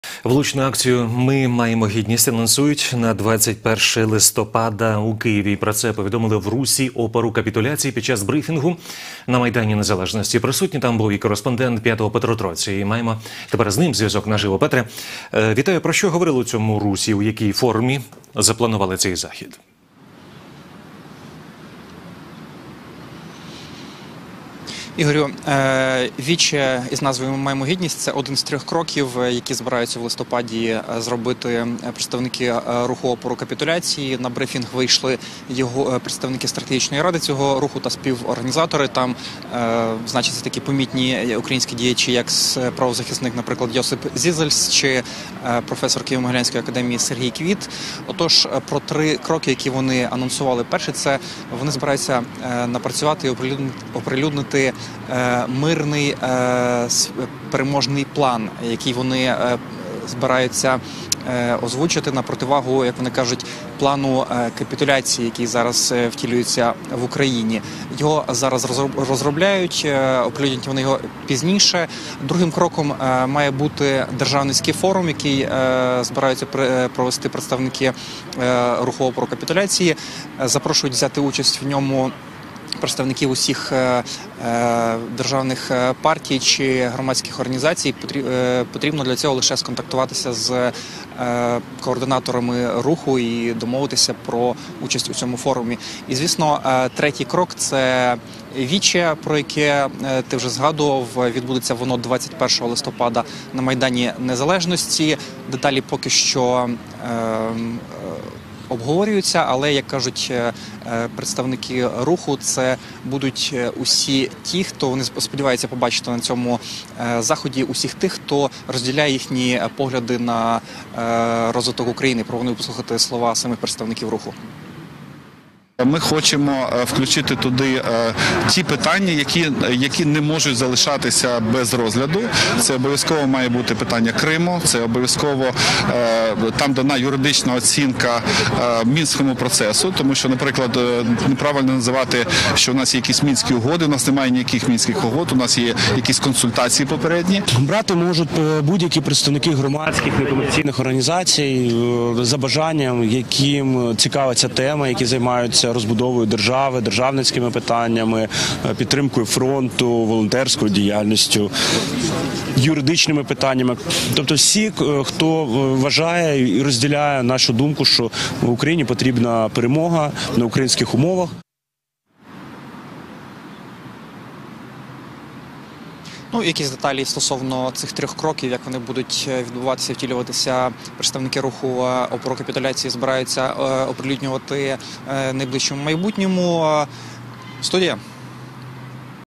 Вуличну акцію "Ми маємо гідність" анонсували представники "Руху опору капітуляції" / включення
Вуличну акцію "Ми маємо гідність" анонсують на 21 листопада у Києві. Про це повідомили в "Русі опору капітуляції" під час брифінгу на майдані Незалежності.